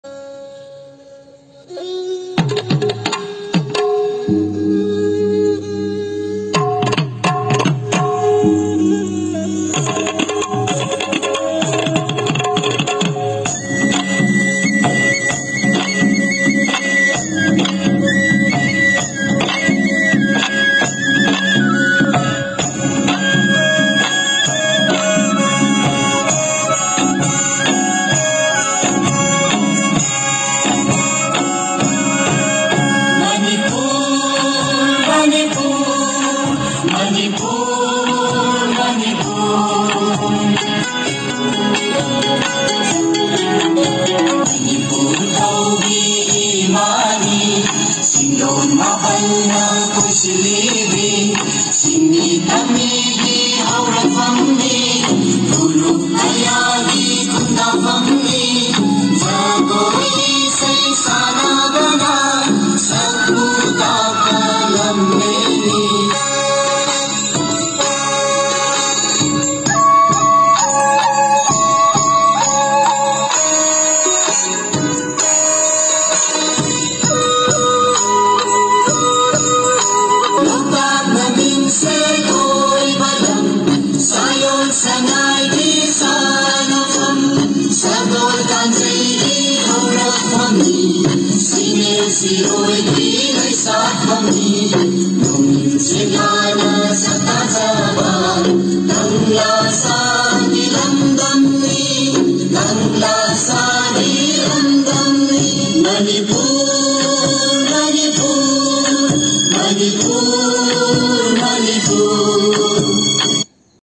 Chandani Devi | Manipur Manipur Contestant 7
Name:- TELEM CHANDANI DEVI MANIPUR  MANIPUR
ADD:- kamranga, jiribam.

Emoinu Channel na meeyamgidamak ngaihaktang oirabasu nungainabagi damak Online acting/dialogue contest sinjari.
Contest asida saruk yabiribasing amadi video asibu yengbiriba mayam pumnamakta Emoina Channel na nungaiba phongdokchari.
Nouna hougatlakliba angang singbu pukning thougatpiduna akhoigi apambada saruk yaminasi.
CHILDREN ARE THE PILLARS OF NATION